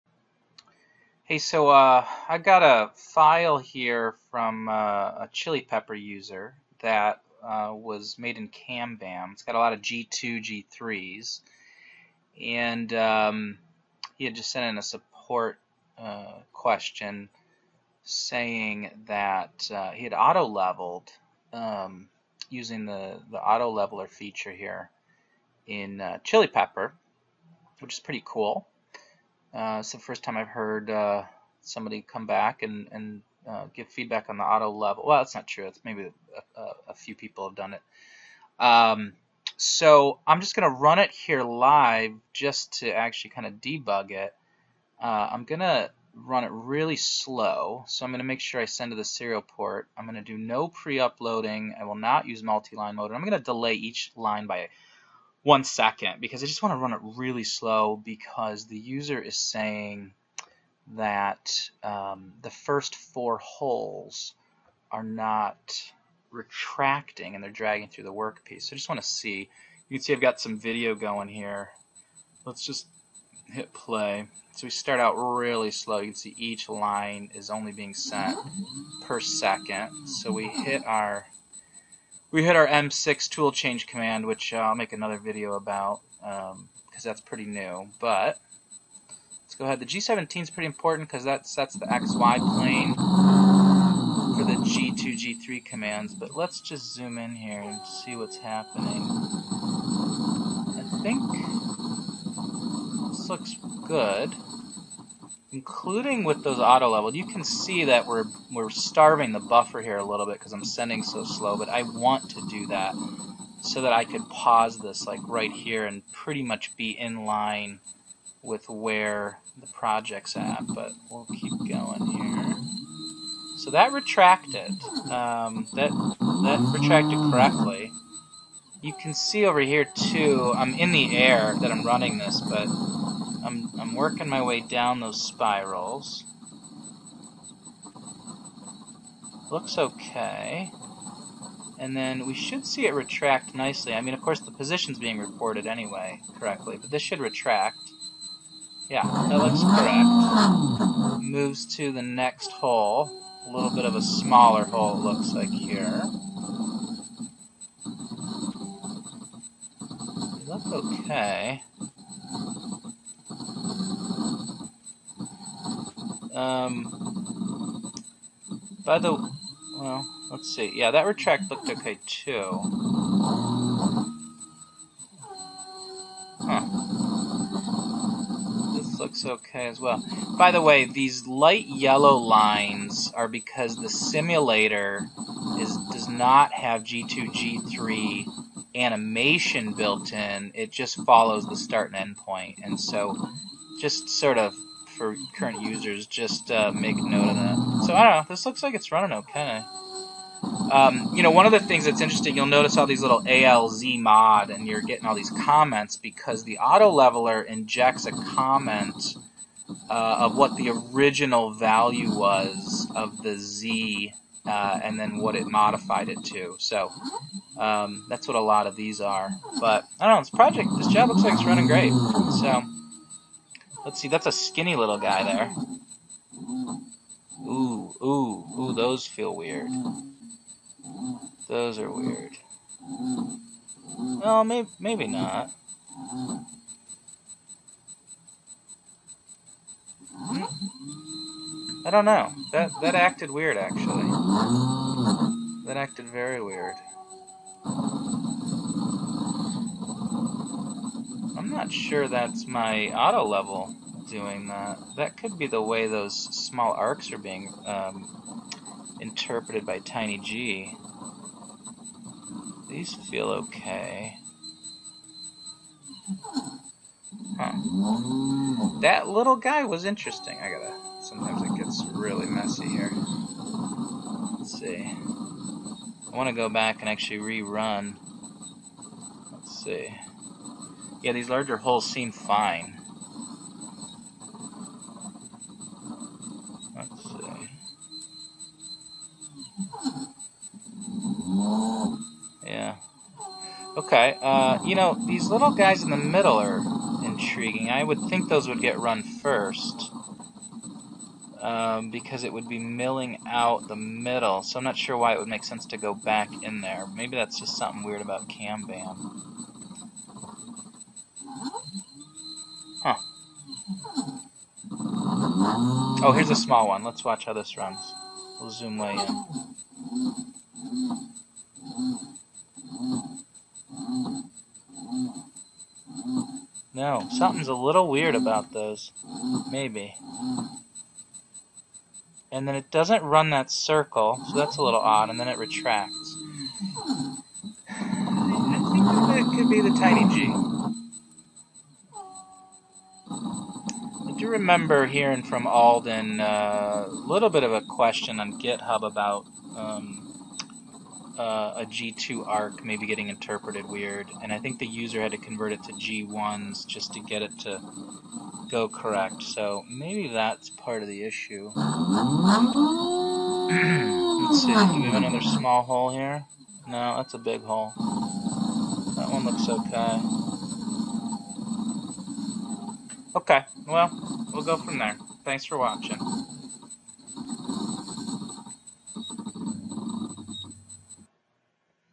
ChiliPeppr Running Auto-Levelled Gcode with G2/G3's in It
Running some Gcode from a ChiliPeppr user to see how the TinyG handles it. The user is having problems with some of the holes seemingly not milling correctly where the end-mill doesn't retract. From my run the tool does retract ok. So, let's see if this video corresponds to the user's experience on their CNC machine.